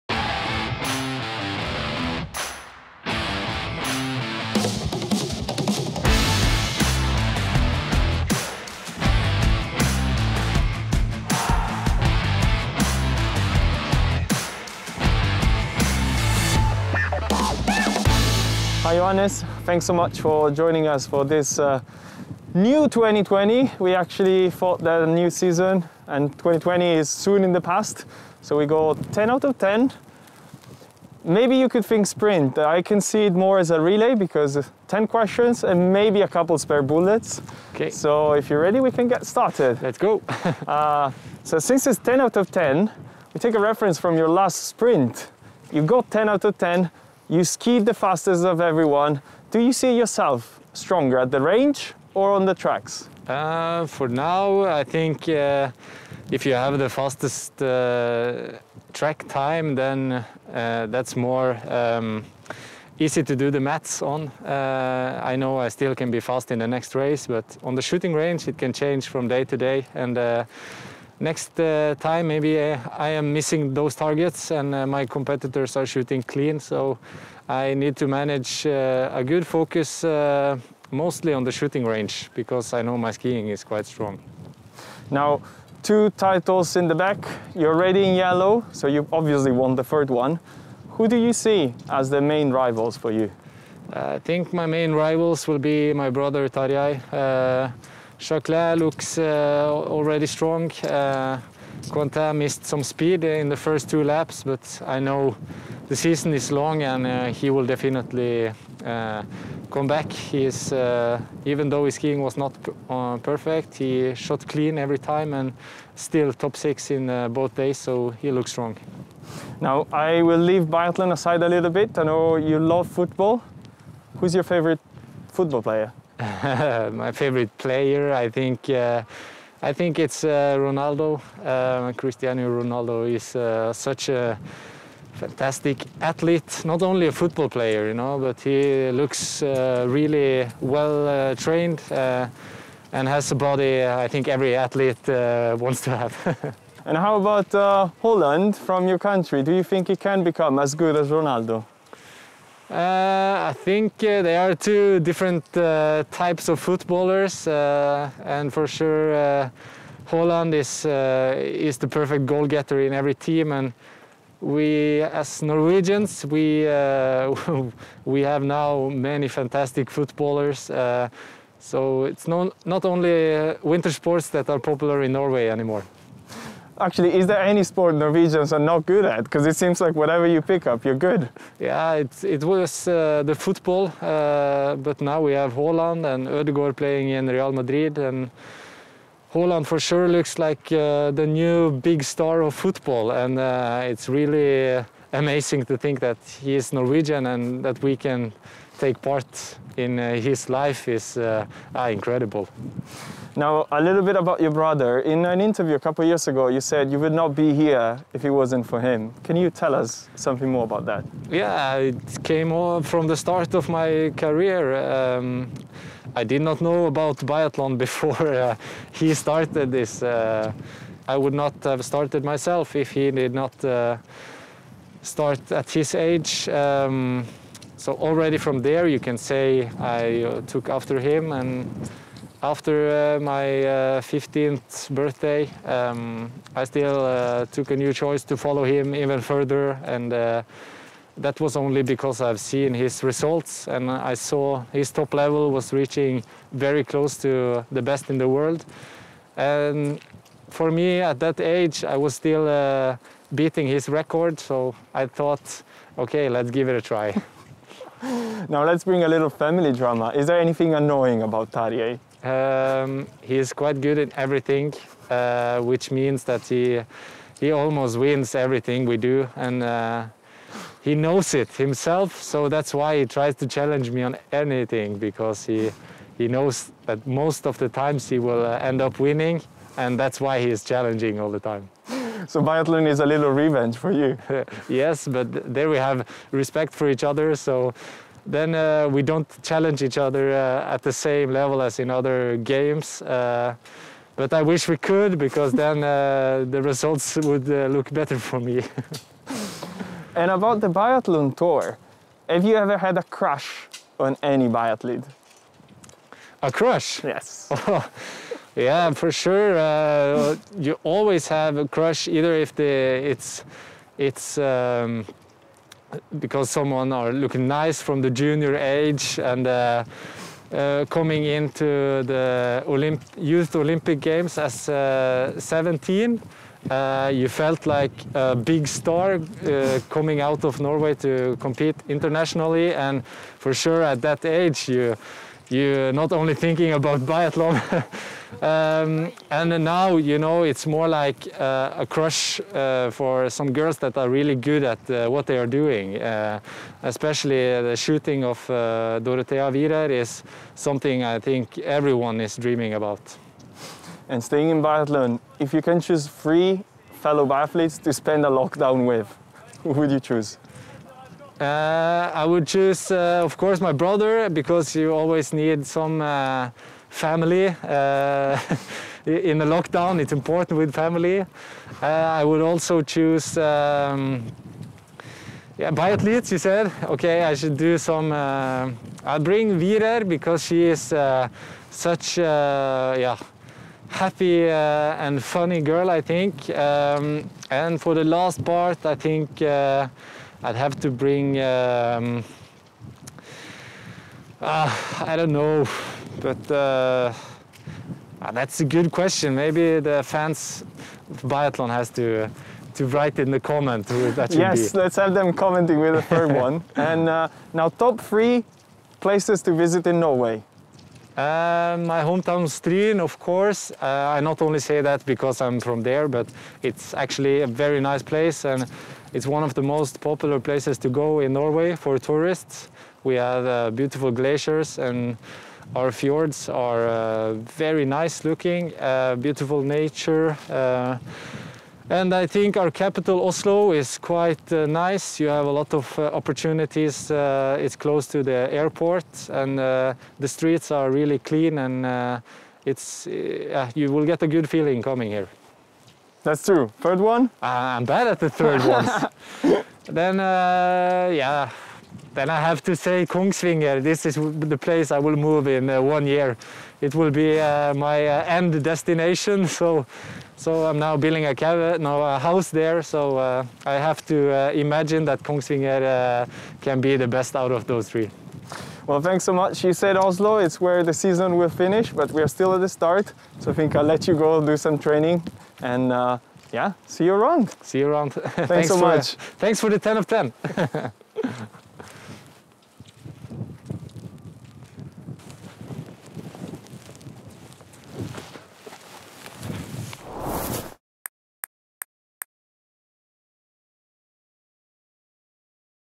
Johannes Thingnes Boe 10/10
Johannes Thingnes Boe is the first guest of our newly launched series "10/10" which replaces last year's highly successful 20/20. With 10 questions fired (and a few spare bullets), the Norwegian reveals himself in this uncut disclosing of his life as a biathlete and more...